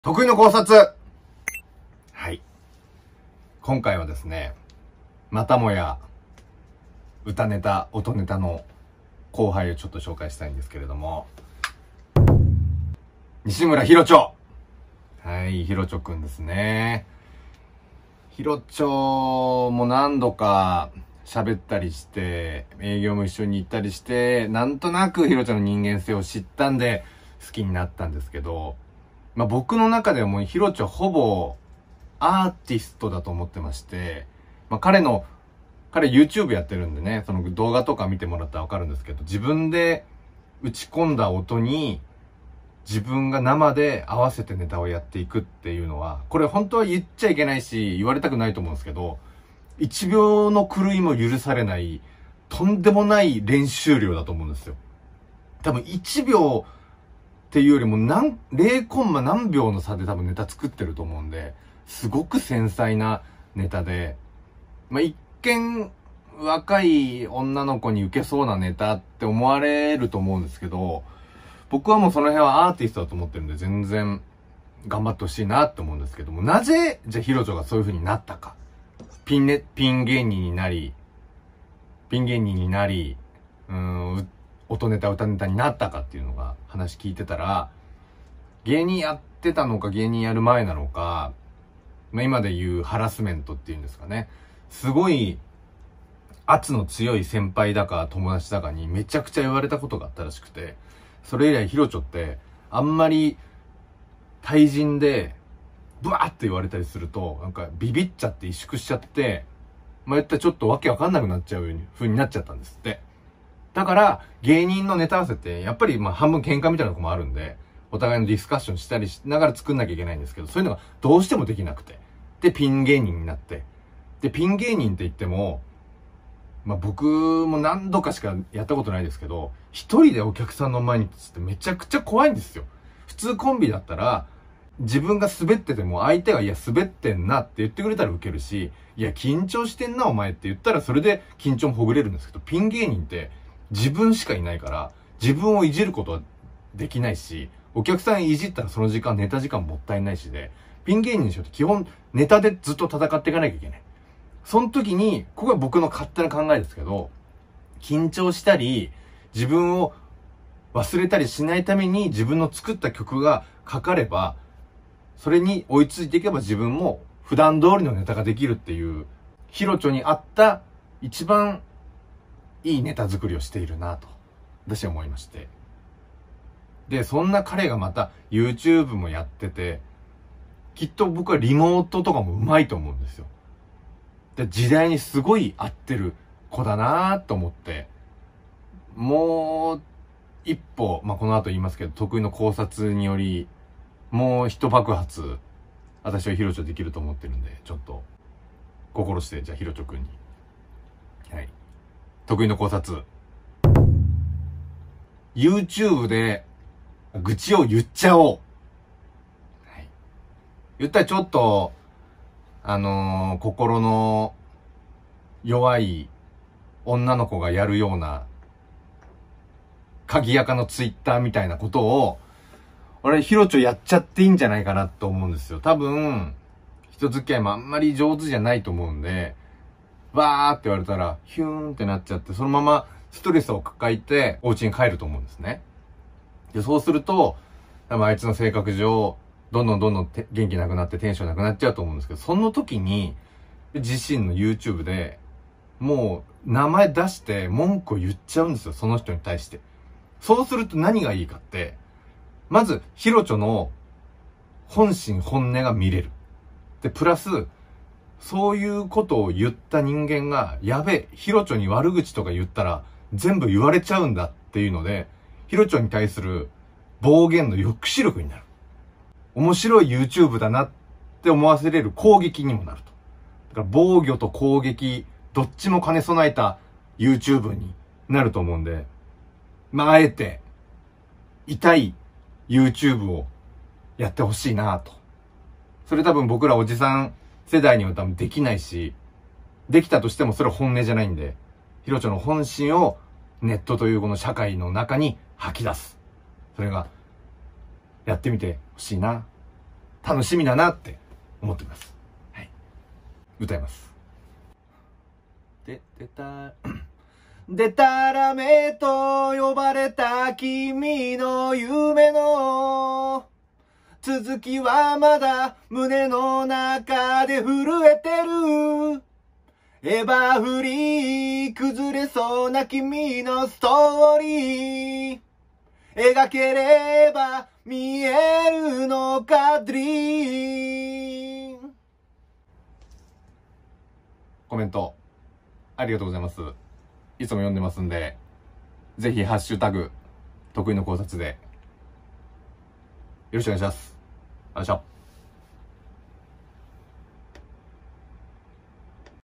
徳井の考察⑧【西村ヒロチョ】
平成ノブシコブシ徳井です！

僕が思う、もっと世間で評価や称賛を受けるべき人や物を紹介したくてこのチャンネルを立ち上げました！

西村ヒロチョさんの事について話しました。